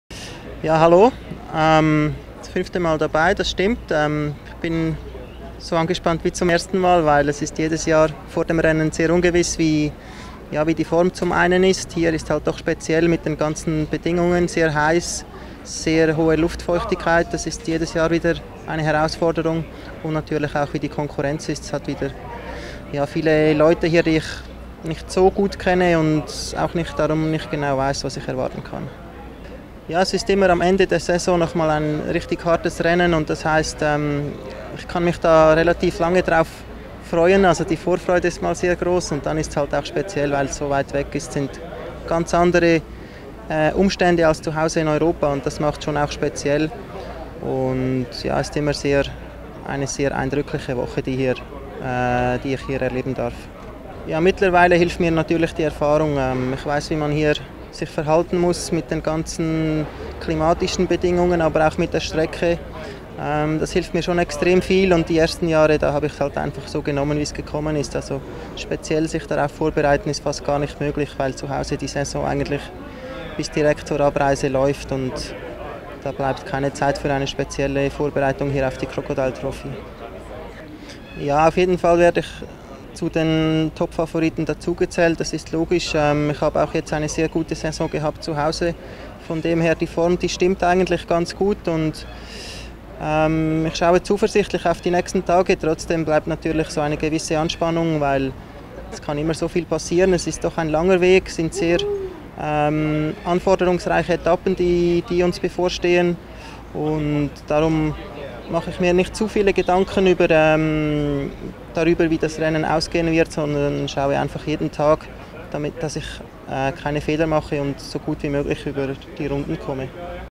Crocodile Trophy 2016 Interview Urs Huber - Race Lead Up Cairns Zoom
Crocodile Trophy - Event Lead-Up
Media Call, Cairns Zoom & Wildlife Dome

OT Urs Huber (SUI, Elite)

0:00:00
Hello, yes, it's true, this is my fifth Crocodile Trophy, but I'm as nervous as at the first time. you never know how you will be handle the conditions here, which are very specific. It's very hot and humid, which is a challenge every year again. Also this year, the competition is hard to judge; there are quite a few riders that I don't know, so I'm not sure what to expect.

0:00:30
[The Crocodile Trophy] is at the end of the season and a final, really hard race, that means I can look forward to it for quite some time, and I can't wait for it to come around each year. What also comes into play is that it's so far away from home, the conditions are different. It's always a really impressive week that I am blessed to experience.

0:00:54
By now my experience at this race helps, I know how you have to behave when it comes to the climate but also the race track. That makes a huge difference. In the first few years I took each day as it came. But you can't really prepare specifically for it, because the racing season at home finishes right before I had to leave. There was no time to do a specific training programme for the Crocodile Trophy.

0:01:21
Of course I'm aware of the fact that I'm counted as one of the race favourites, that's clear. Plus, I've had a really good season back home. So, in that sense I'm in good shape. I'm confident about the next few days. But of course, there is always a bit of stress, because so many things can happen - it's a long race and the stages ahead are very demanding, that's why I try not to think a lot about how the final race result might look like, but try to do my best each day.